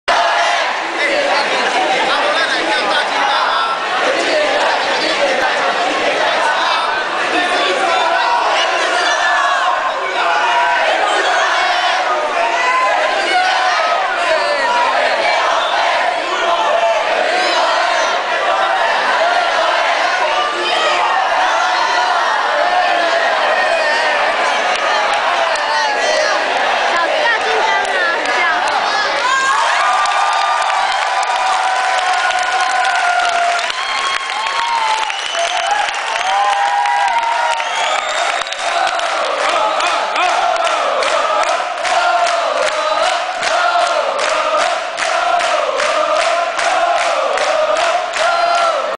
070726 Activity at the dinner table
童軍的活力也可以從餐桌上表現出來, 來瞧瞧外國夥伴所帶來的活動吧!!!
Let show the scouting spirit at dinner time!!!